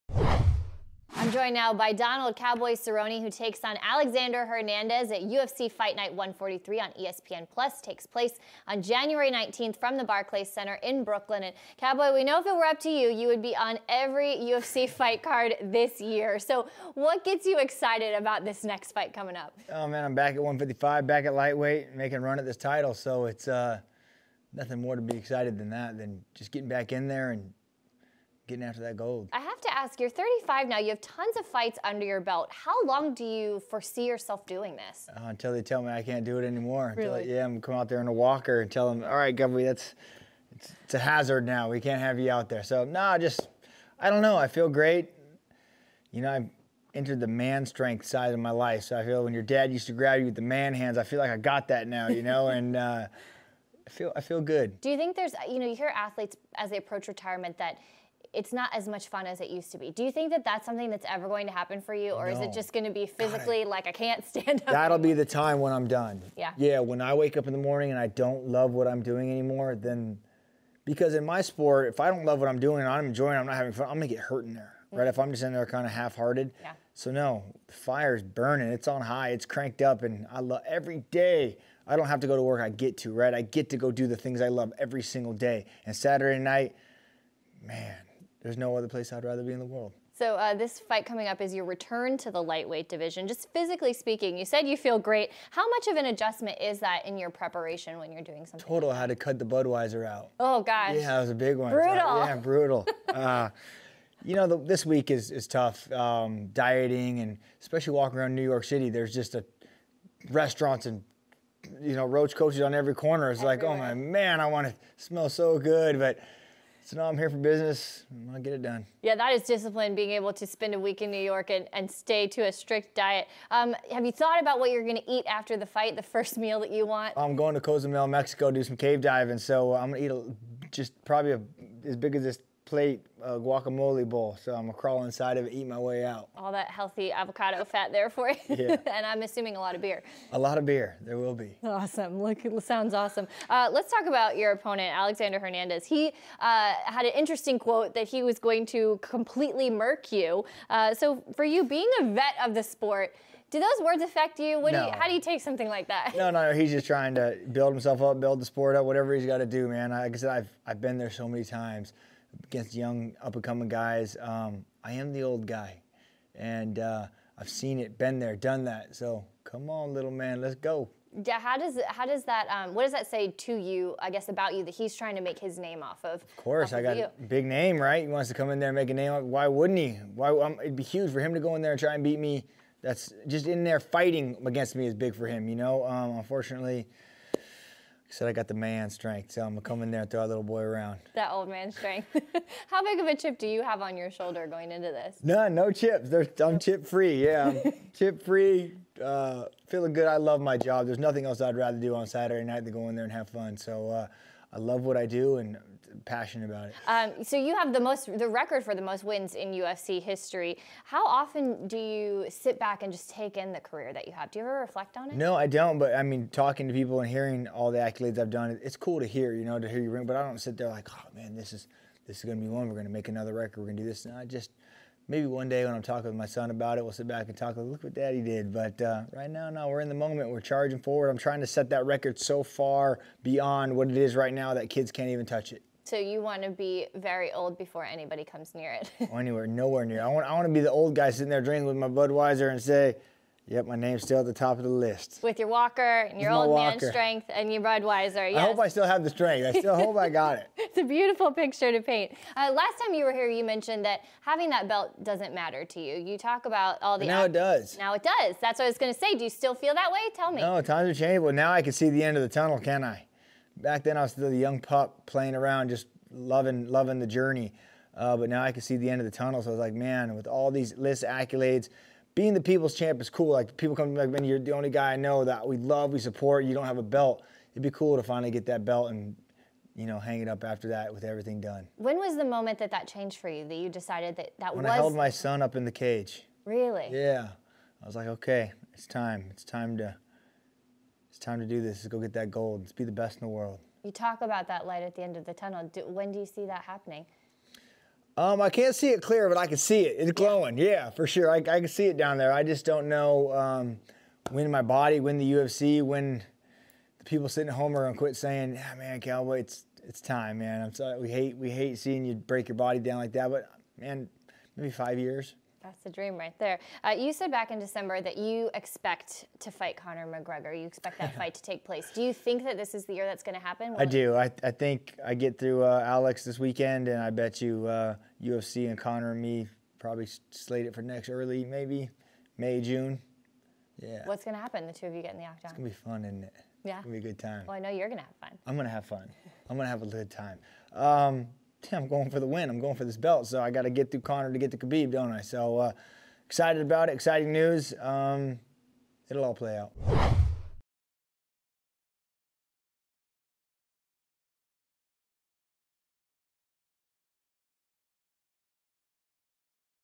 UFC Fight Night: Cowboy Cerrone responds to Alexander Hernandez, Conor McGregor | SPORTS ILLUSTRATED
At UFC Fight Night 143 Cowboy Cerrone takes on Alexander Hernandez. He joined Si Now to share his goal of becoming lightweight champion, fighting Conor McGregor and how long he plans on staying in the fight game.